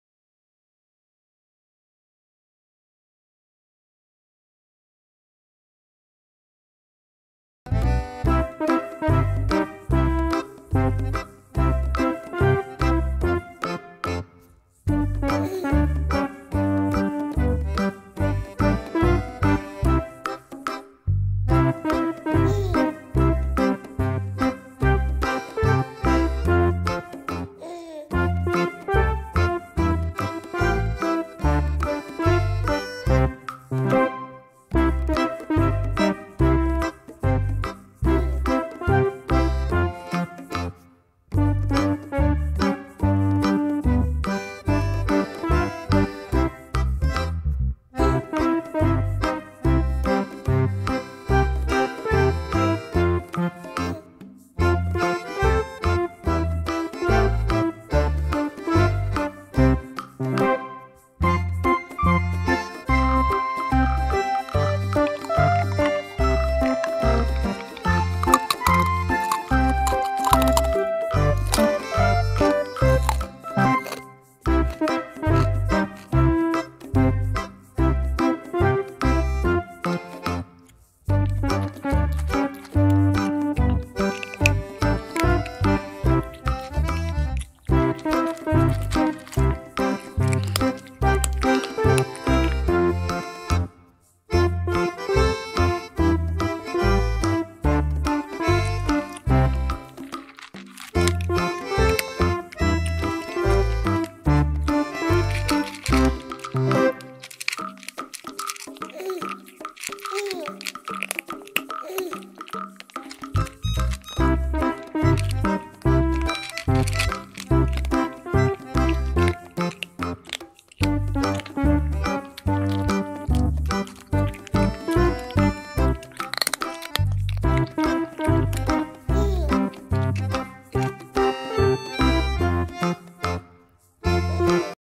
Thank the Lord Hamster Luckily Found Some Mini Apple to Fill in His Pouch Deliciously
Hello Everyone!